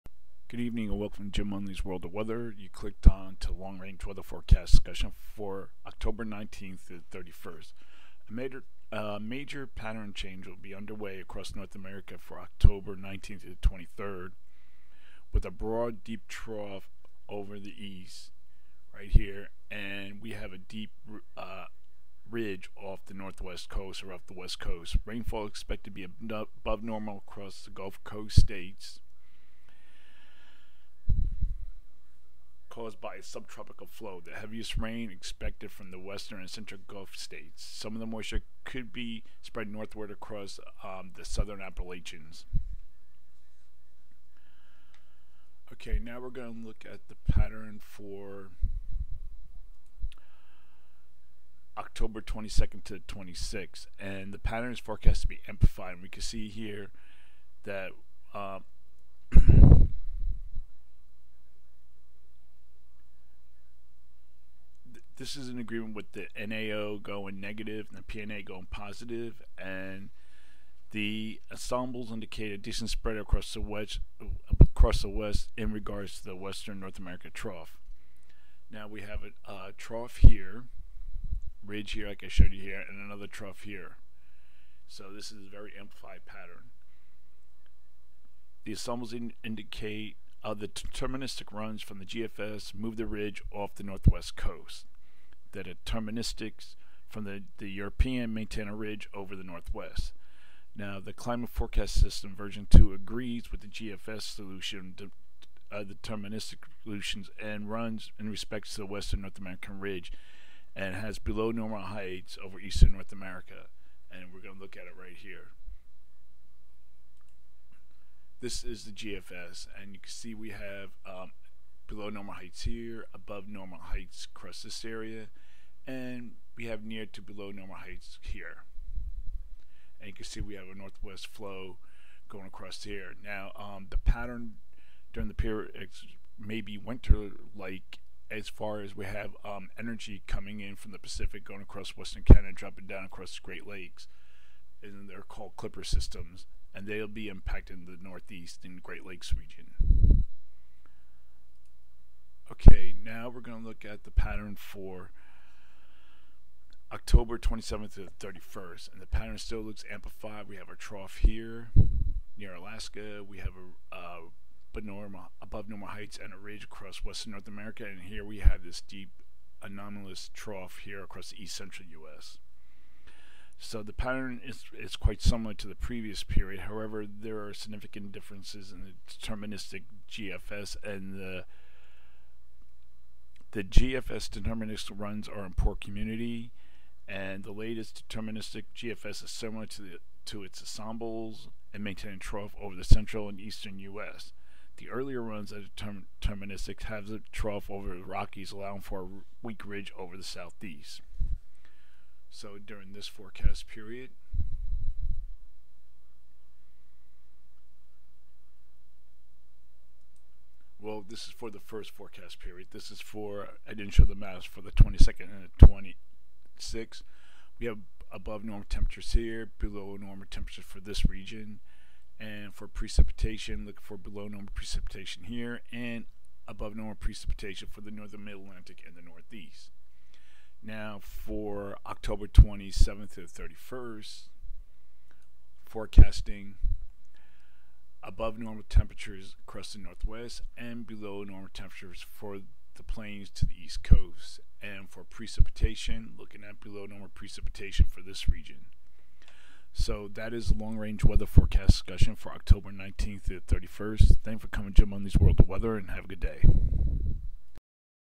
Long Range Weather Forecast Discussion Oct 19 31
A pattern change is expected.  Warm in the West and cool in the East.  Wet  across the gulf coast states.